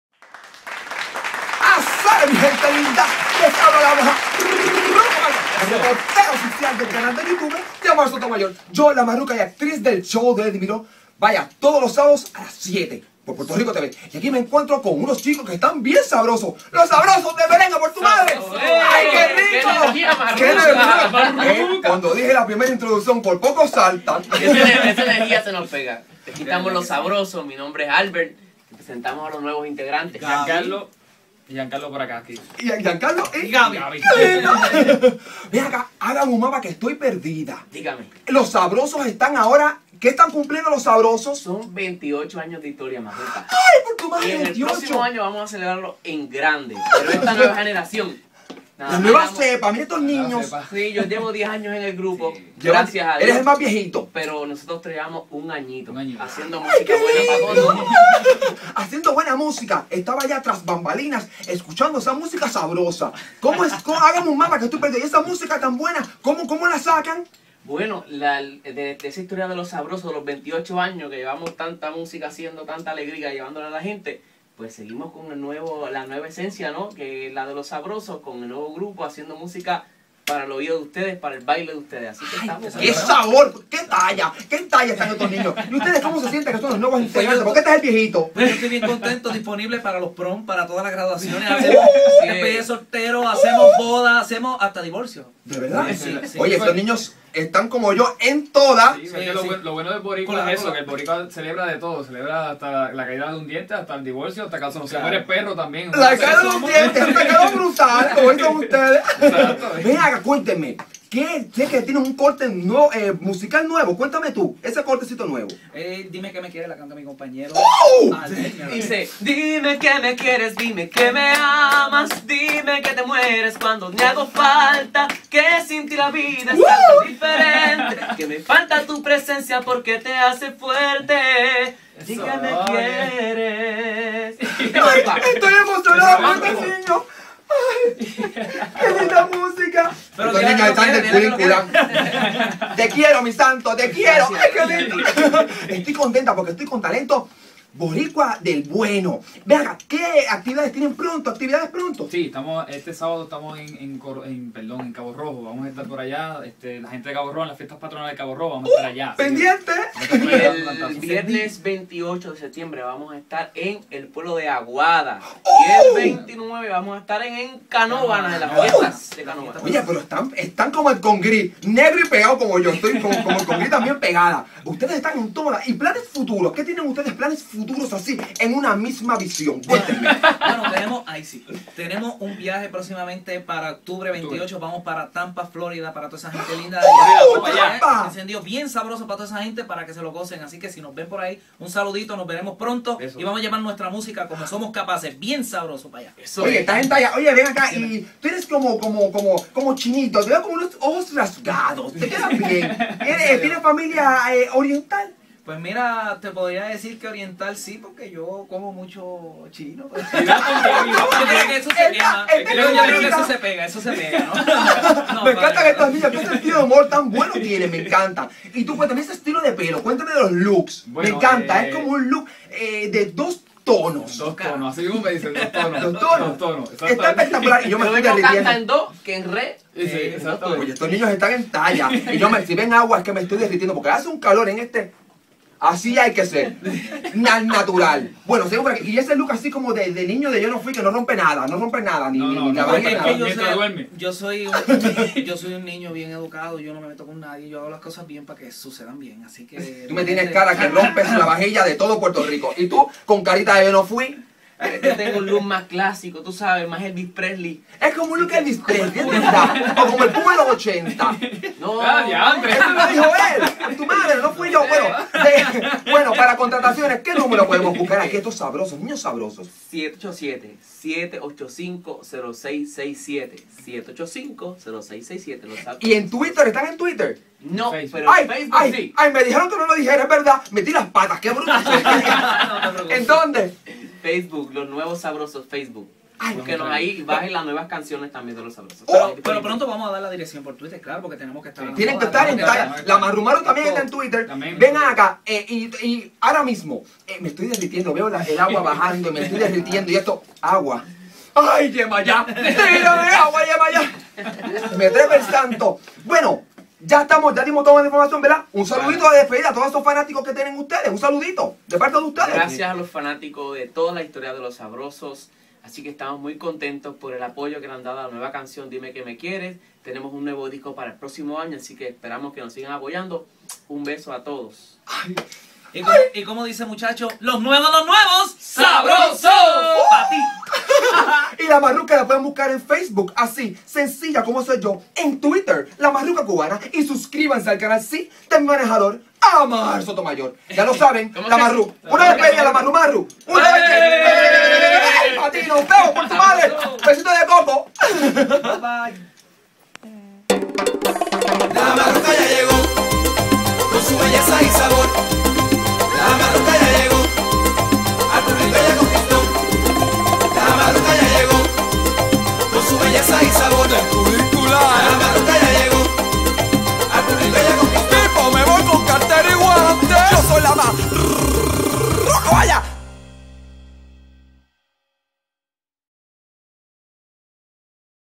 Los Sabrosos Del Merengue en entrevista con La Marruca
La Marruca, la reportera del canal de Youtube de Amar Sotomayor y actriz del "Show de Eddie Miró", entrevista a la Los Sabrosos Del Merengue.
Facebook & Twitter: Amar Sotomayor
Facebook: La Marruca Twitter: lamarrucacubana
Show de Eddie Miró PRTV Canal 6 sábados 7pm(En EU Wapa América y PR Network)